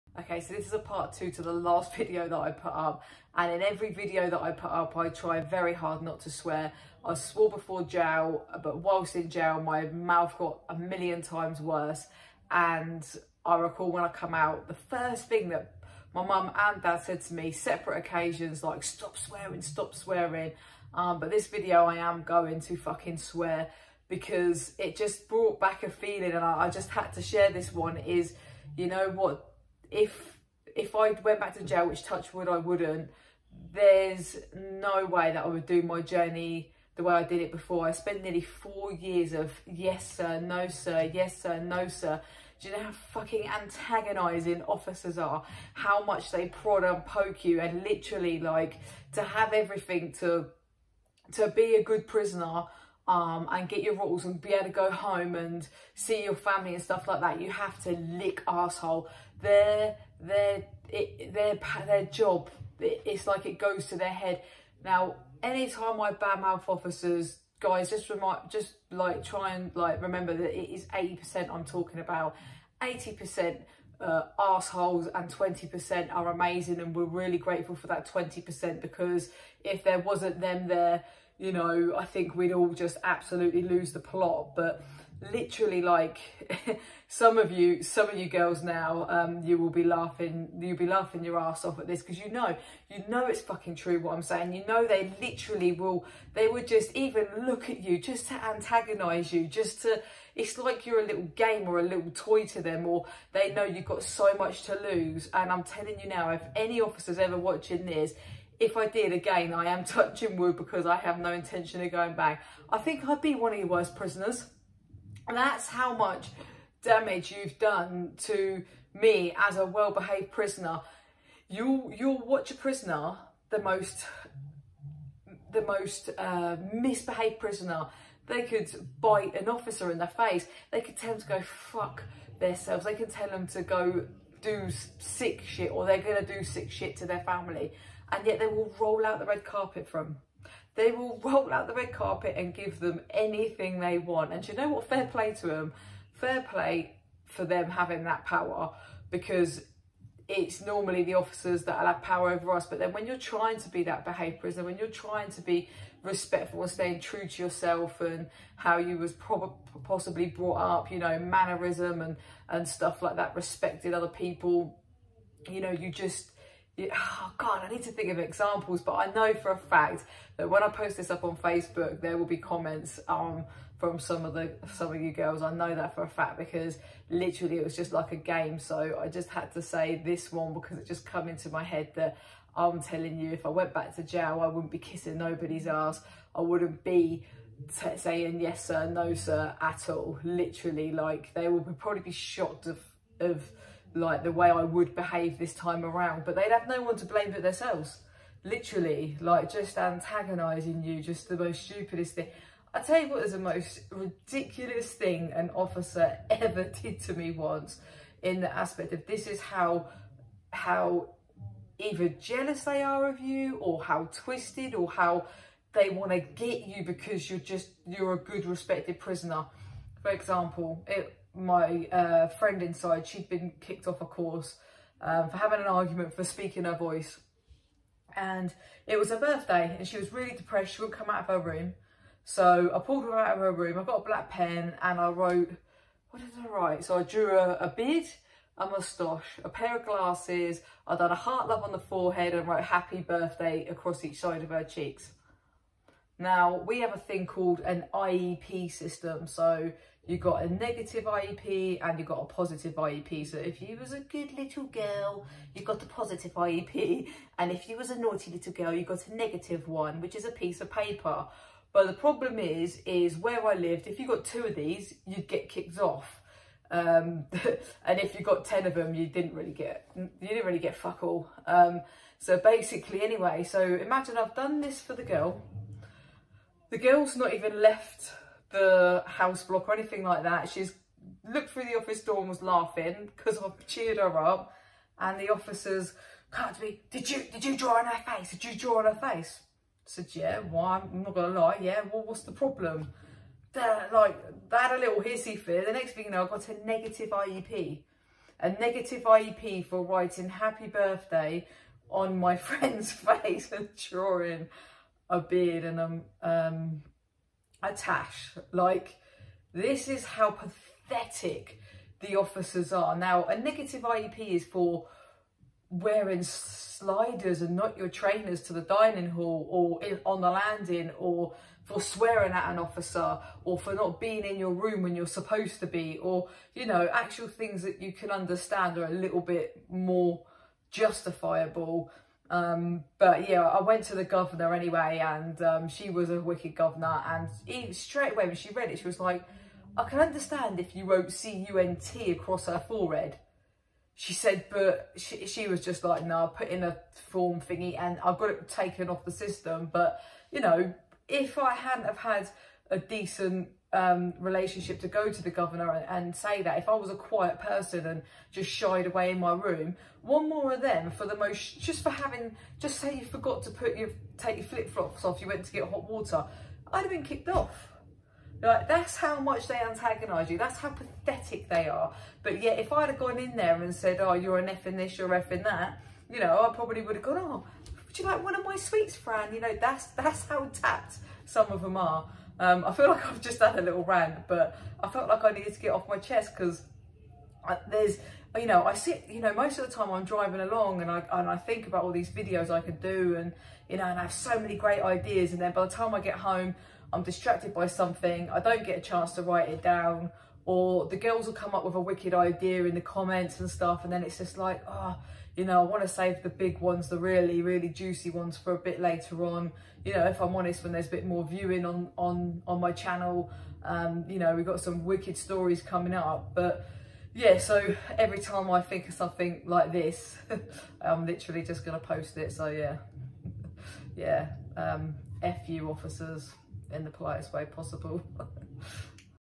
Part 2 to my last uploaded video.
How different would I behave if I did go back to jail and why? This is just a small example as to why I would do my sentence different to the last time around. Please excuse any swearing.